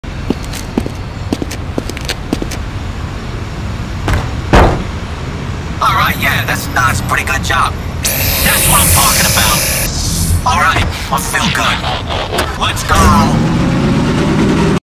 (SFM) Bumblebee change into Earthmode
Bumblebee found yellow car, and he changed into earthform